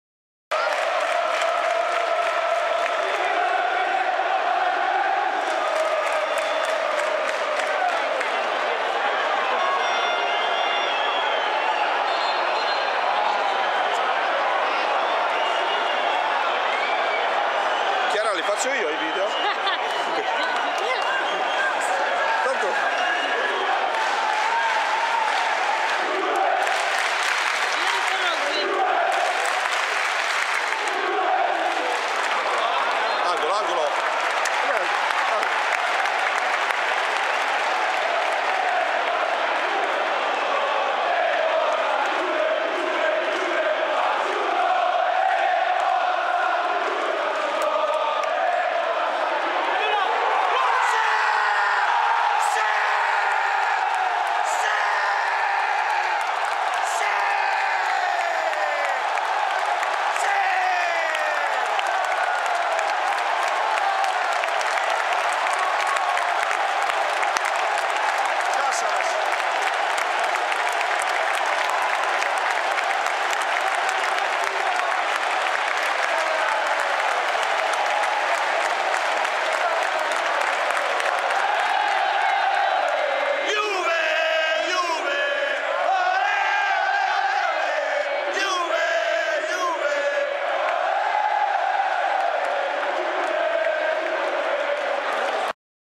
Chievo Vs JUVENTUS AutoGoal Biraghi 0-1
Serie A  1^  giornata 7' 1° Tempo  Stadio Bentegodi Verona 30/08/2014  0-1(Aut.Biraghi)